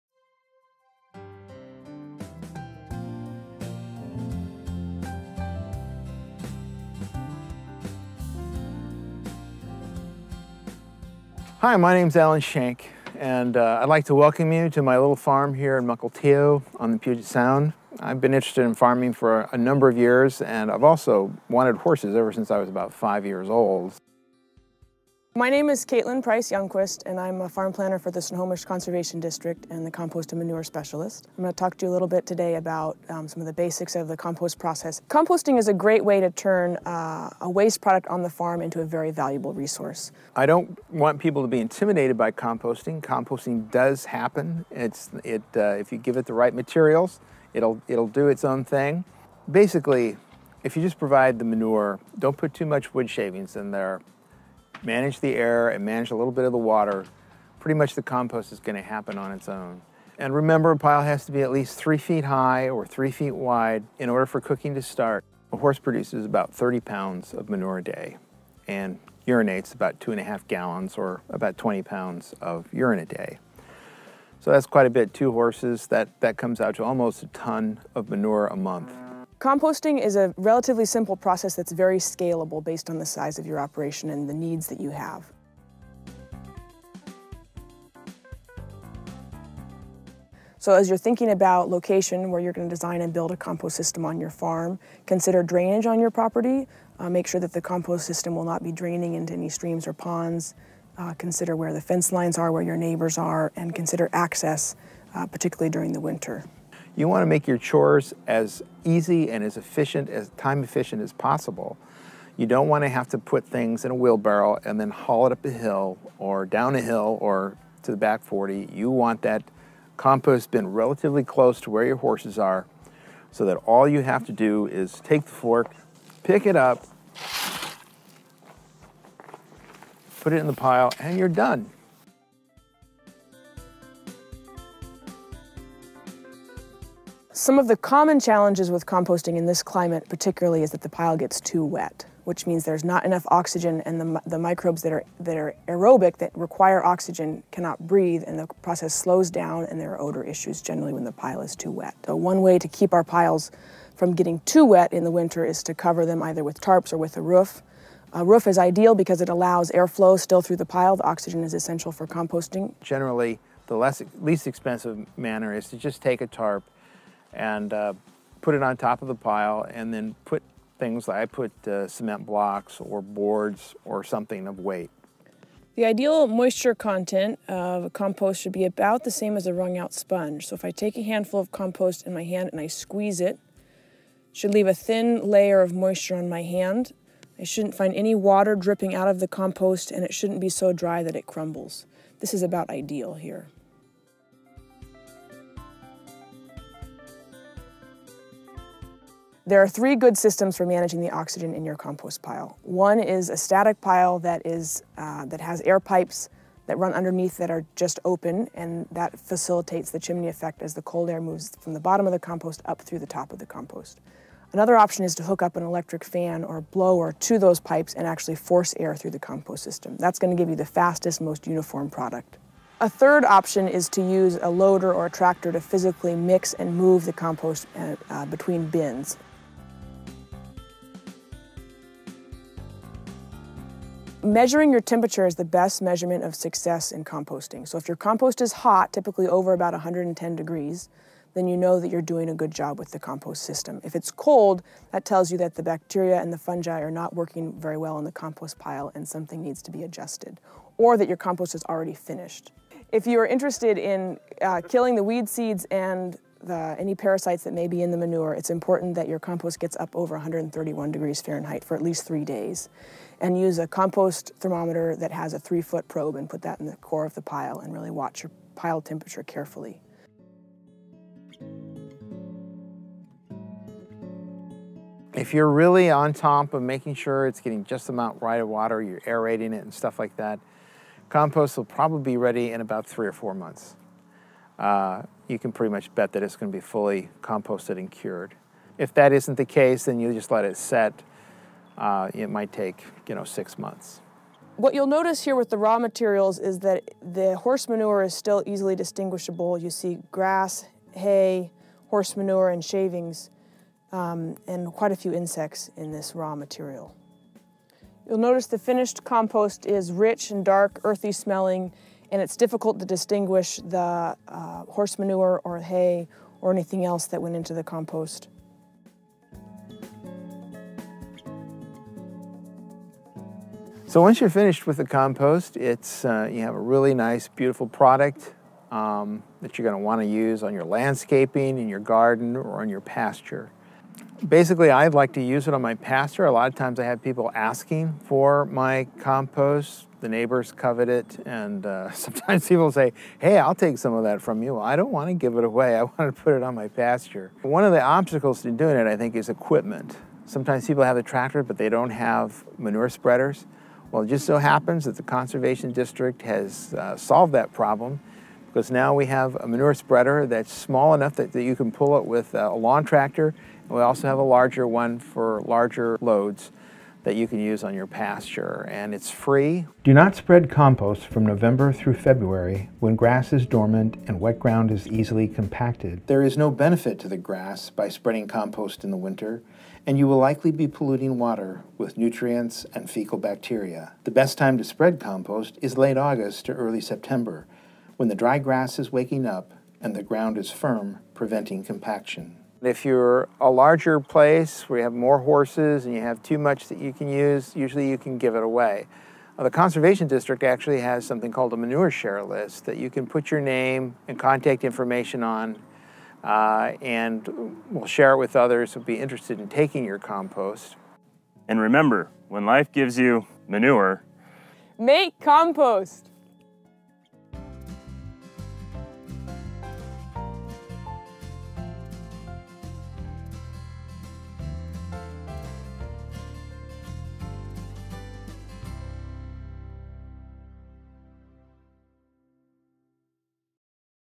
Sound Horsekeeping: Composting Manure
In this 7-minute video you'll learn the basics of composting, common challenges and solutions, and how to put your compost to use once it's finished. This video features farm planners Alan Shank and Caitlin Price-Youngquist from the Snohomish Conservation District.